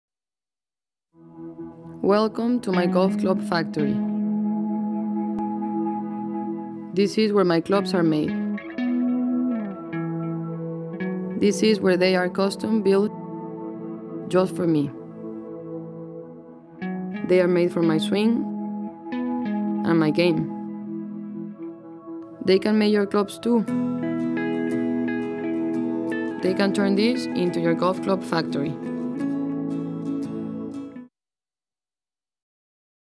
Lorena Ochoa's golf factory
a tour of PING with Lorena.